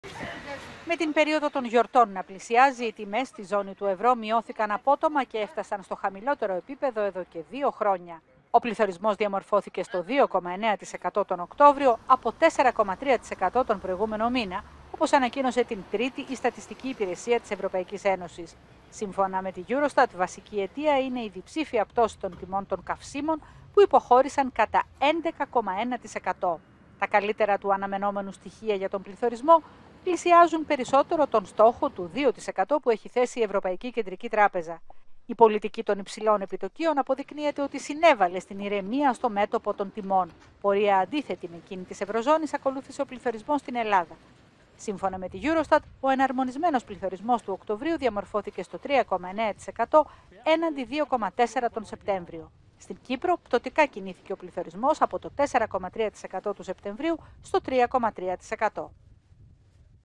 Πληθωρισμός: Αύξηση στην Ελλάδα, μείωση στην Κύπρο, σε χαμηλό διετίας στην Ευρωζώνη
Πώς κινήθηκε ο δείκτης τιμών καταναλωτή τον Οκτώβριο, σύμφωνα με τις προκαταρκτικές εκτιμήσεις της Eurostat